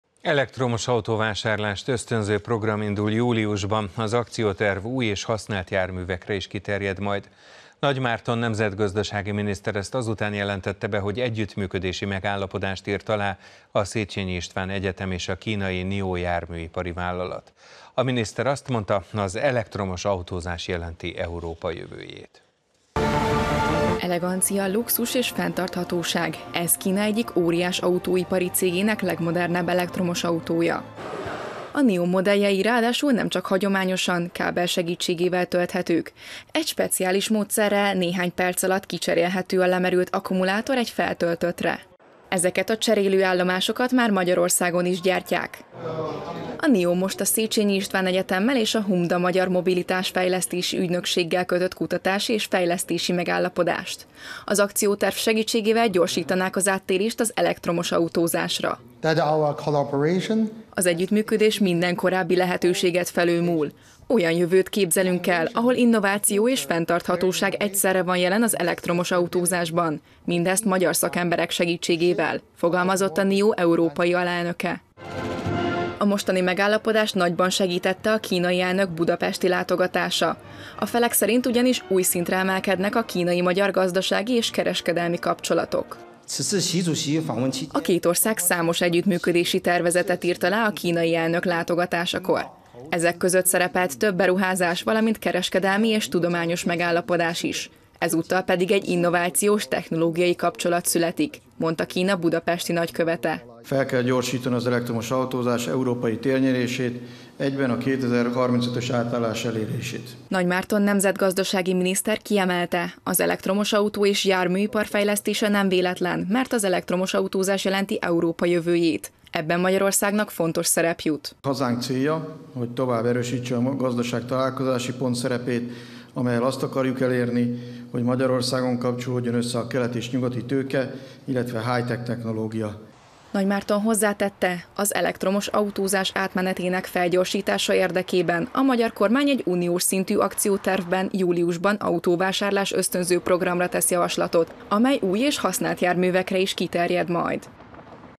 Elektromos autóvásárlást ösztönző akcióterv indul
Elektromosautó-vásárlást ösztönző program indul júliusban. Az akcióterv új és használt járművekre is kiterjed majd. Nagy Márton nemzetgazdasági miniszter ezt azután jelentette be, hogy  együttműködési megállapodást írt alá a Széchenyi István Egyetem és a kínai NIO járműipari vállalat. A miniszter azt mondta: az elektromos autózás jelenti Európa jövőjét.